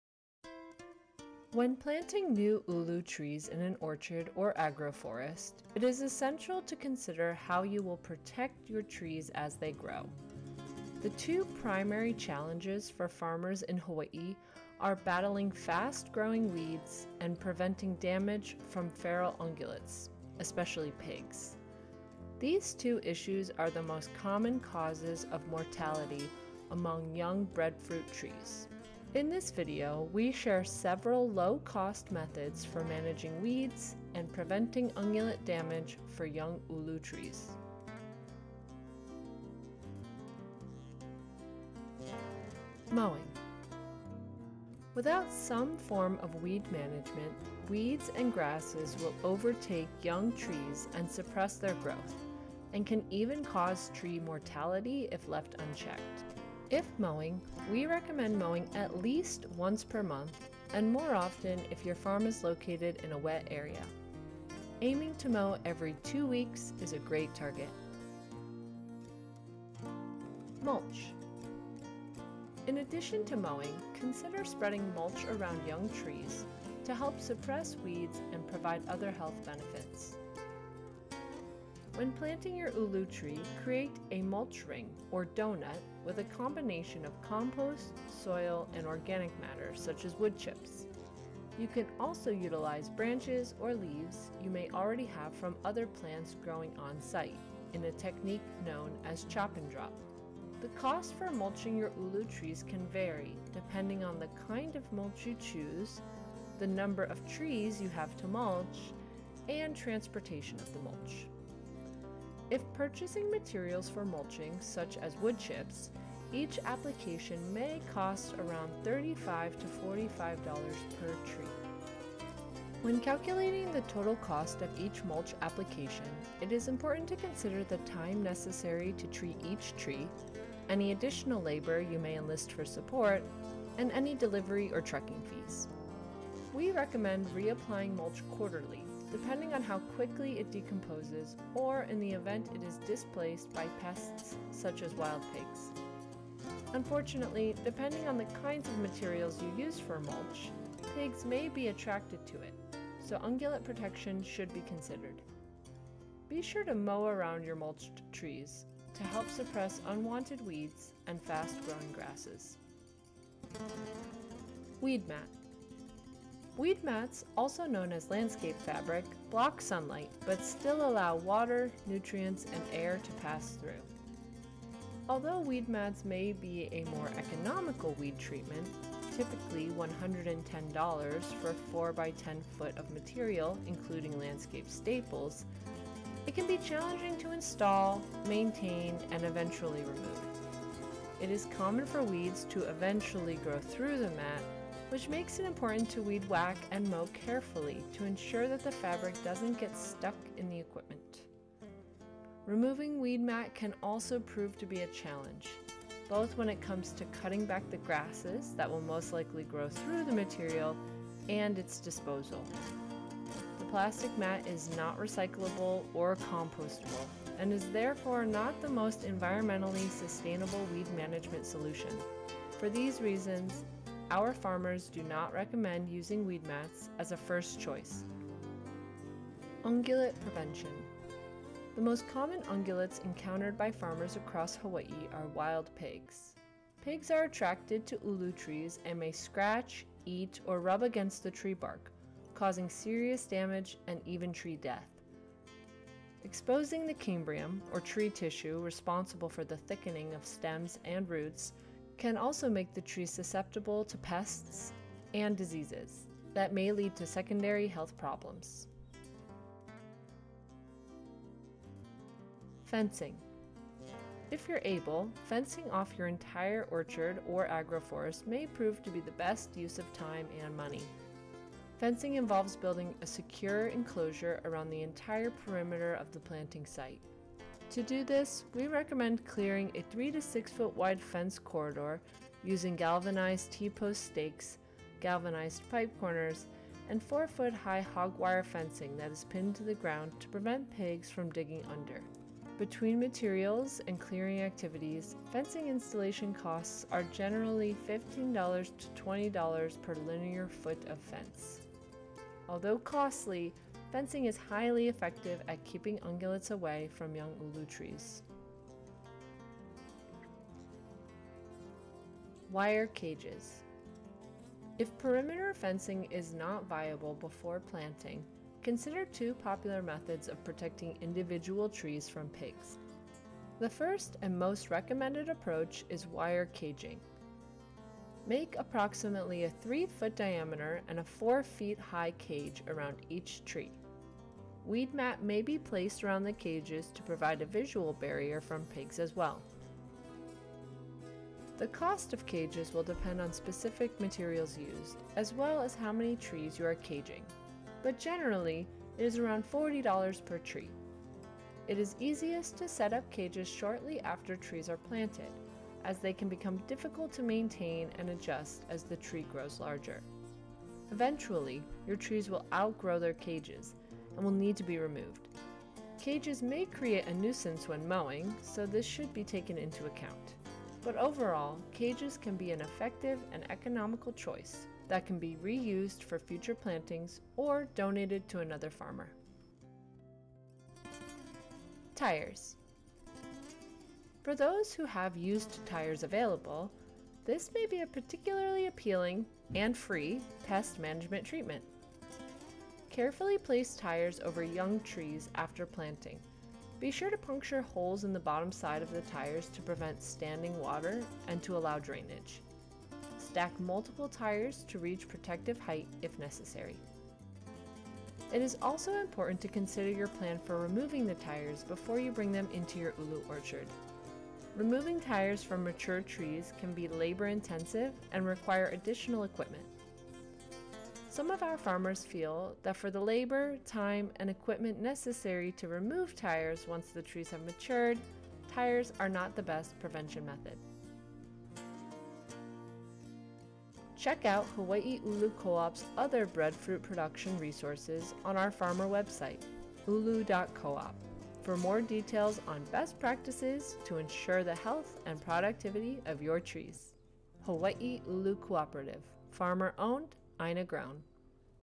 Weed & Ungulate Management for Young ʻUlu Tree Establishment
From mowing to weedmat, cages to tires, this video explores some of the management methods that Hawaiʻi ʻUlu farmers are using to mitigate the impacts of invasive weeds and pigs when trying to establish new ʻulu tree plantings.